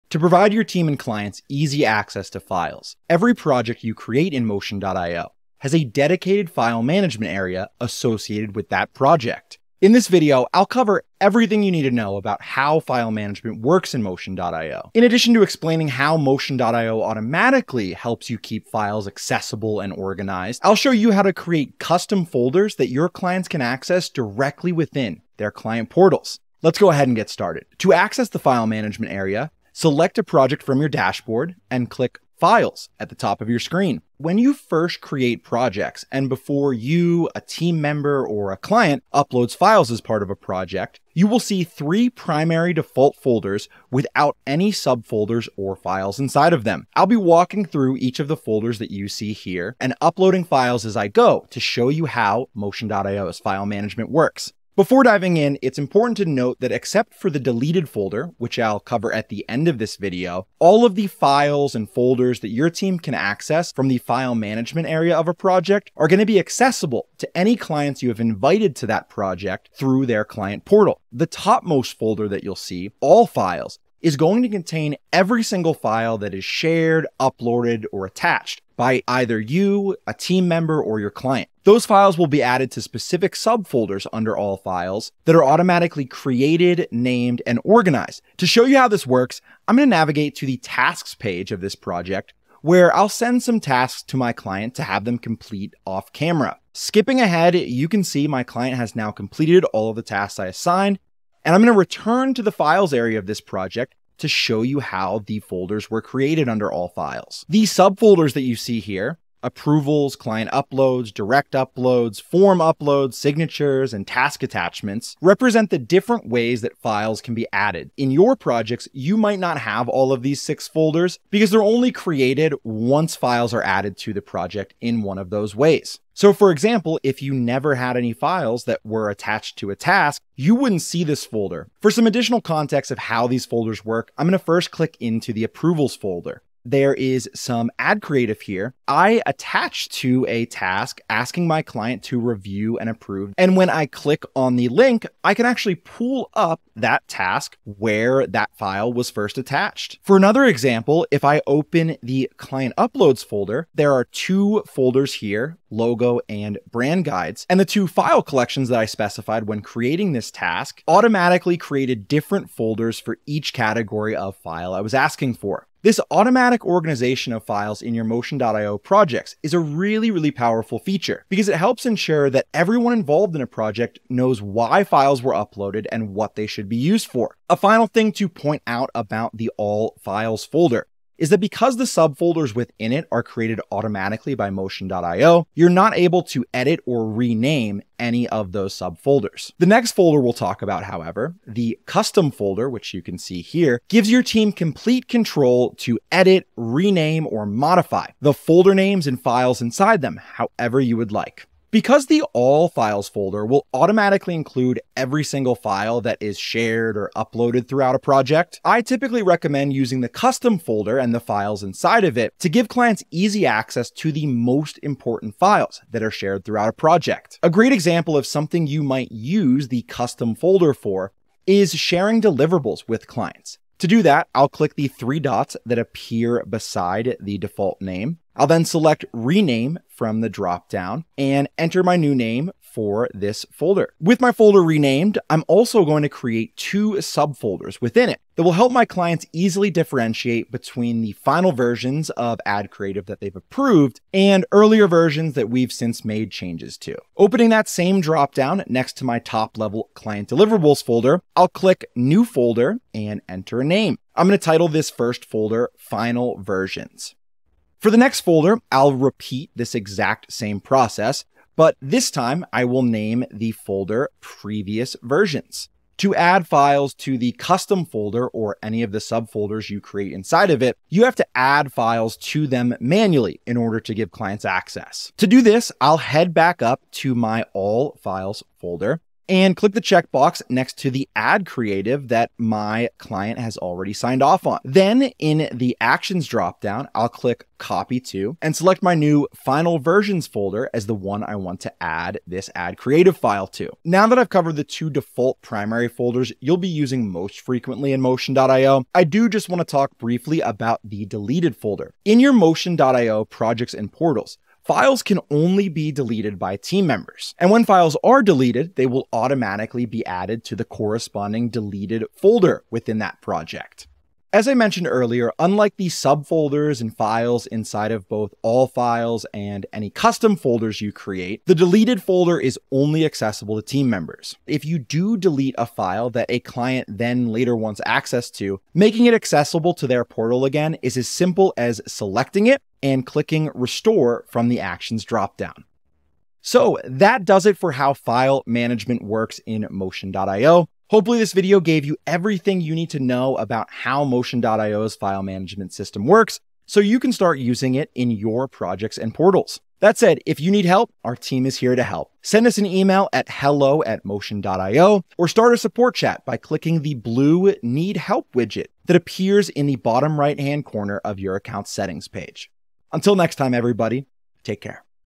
How File Management Works in Motion.io Client Portals & Projects
*NOTE* Since this video was published, we've released a handful of updates to improve to Motion.io's file management functionality. For up-to-date documentation on how file management works in Motion.io, consult the following guide

To provide your team and clients easy access to files, every project you create in Motion.io includes a dedicated file management area. In this video, co-founder Sam Chlebowski explains how file management works in your Motion.io projects and how you can create, customize, and modify the files and folders customers can access within their client portals